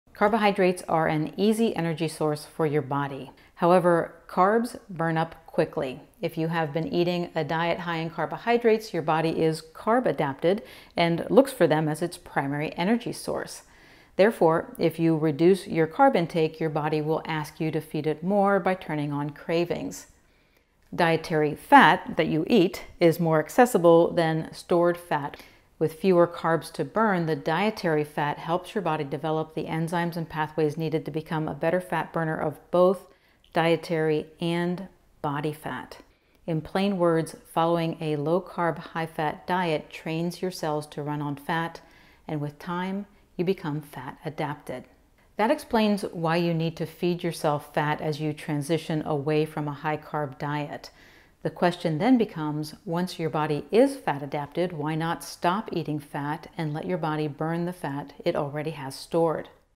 Getting Fat-Adapted (Carb-Burner vs. Fat-Burner)
"Carbohydrates are an easy energy source for your body. It is a much easier source than pulling fat out of storage. However, carbs burn up quickly. If you have been eating a diet high in carbohydrates, your body is "carb-adapted" and looks for them as its primary energy source. Therefore, if you reduce your carb intake, your body will ask you to feed it more by turning on cravings. Dietary fat - that you eat - is more accessible than stored fat..."